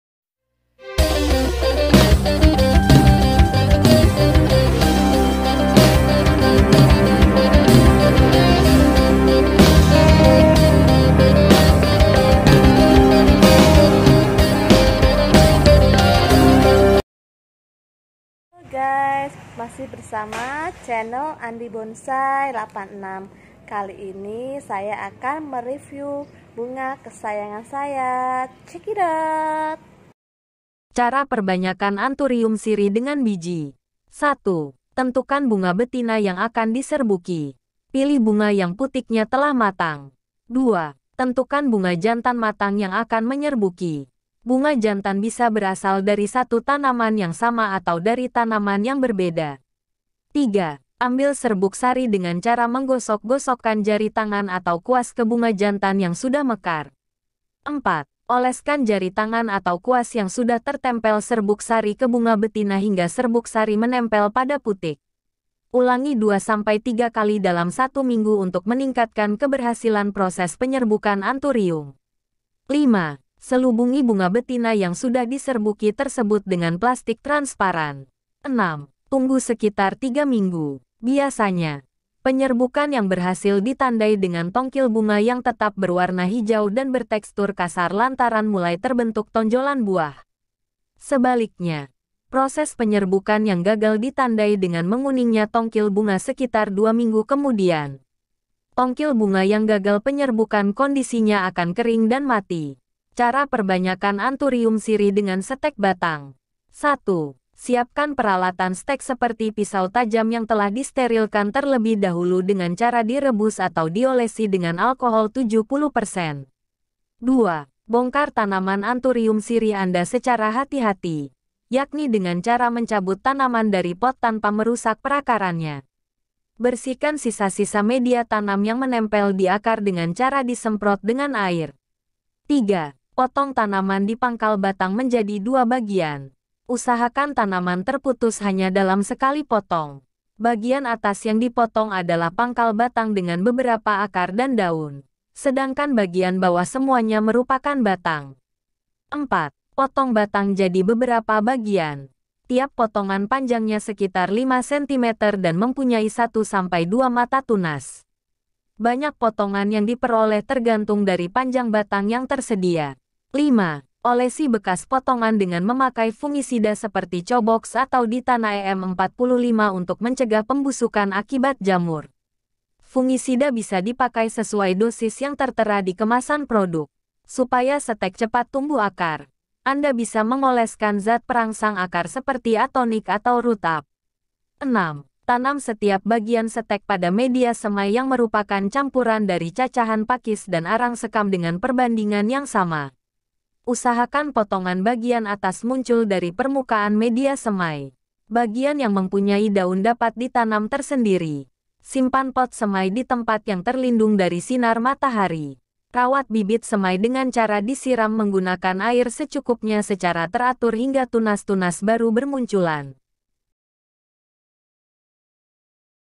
2 Cara Budidaya Anthurium Sirih
2 Cara Budidaya Anthurium Sirih.
Tampilan sosok tanaman hias jenis ini terlihat kekar dan terkesan eksklusif. Tampilannya yang anggun memberikan kesan mewah. Varian ini juga sempat booming beberapa tahun silam, harganyapun sempat mencapai angka yang sangat fantastis. Setelah kepopulerannya sempat turun, kini Anthurium Sirih mulai kembali naik daun masih dengan harga premium namun tidak semahal dulu. Bentuk daunnya yang unik dengan aneka variasi yang sangat indah menjadi daya tarik tersendiri. Kami jual anturium sirih ukuran standar dalam polibag 25cm dengan tinggi 20-60cm. Tersedia juga.